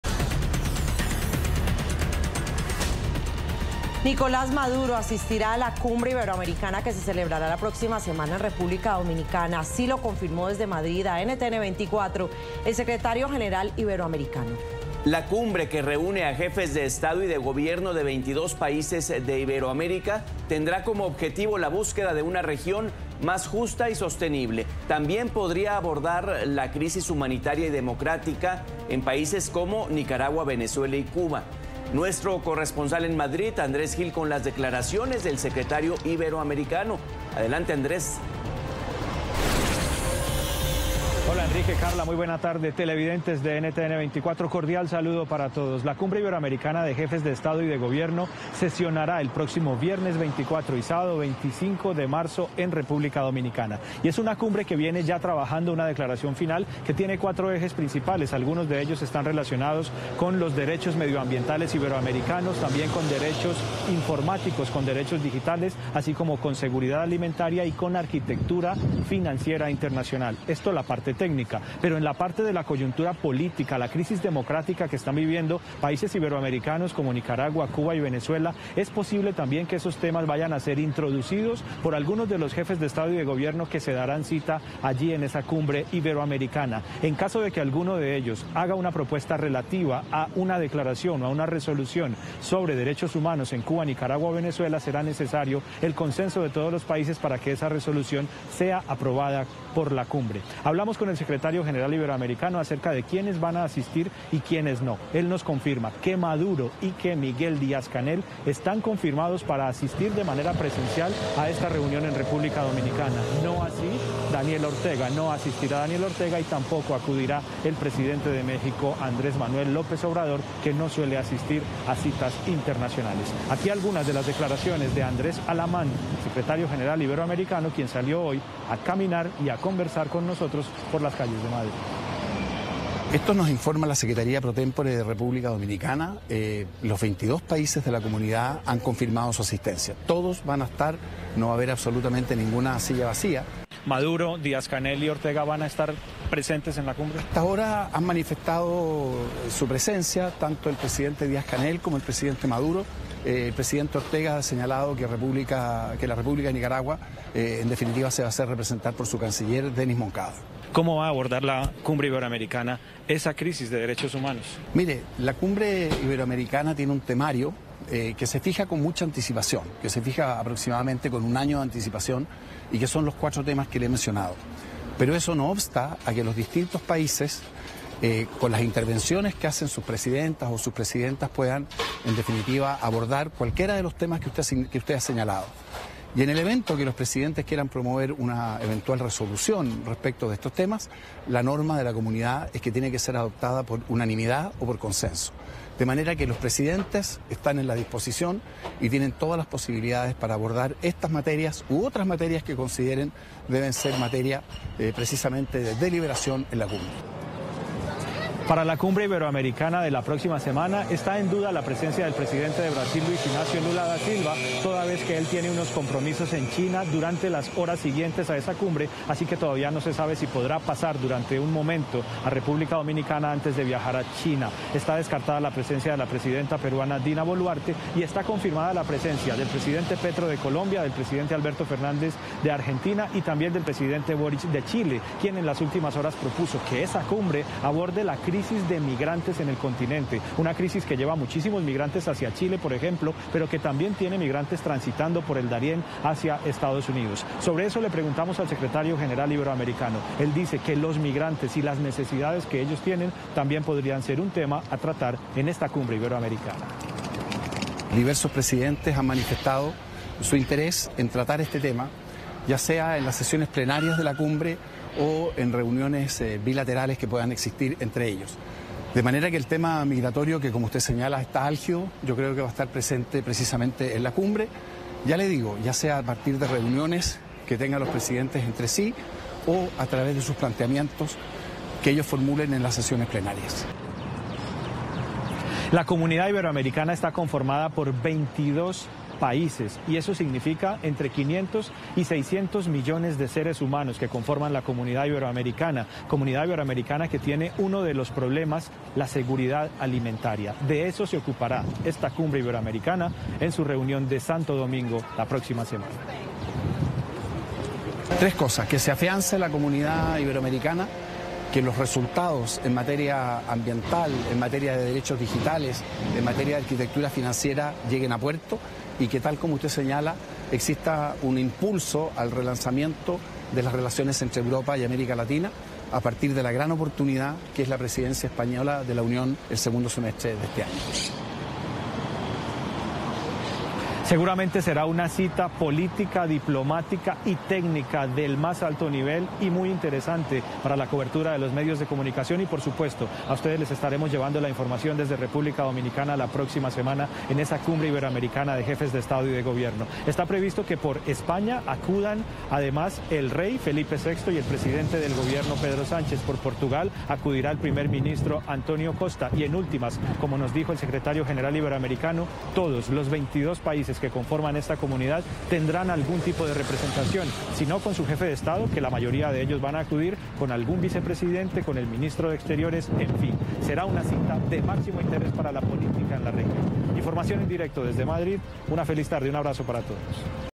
Maduro asistirá a la Cumbre Iberoamericana en República Dominicana
Nicolás Maduro, líder del régimen en Venezuela, asistirá a la Cumbre Iberoamericana que se celebrará la próxima semana en República Dominicana, así lo confirmó desde Madrid el  Secretario General Iberoamericano.

El encuentro que reúne a jefes de Estado y de Gobierno de 22 países de Iberoamérica, tendrá como objetivo la búsqueda de una región "más justa y sostenible".

También podría abordar la crisis humanitaria y democrática en países como Nicaragua, Venezuela y Cuba.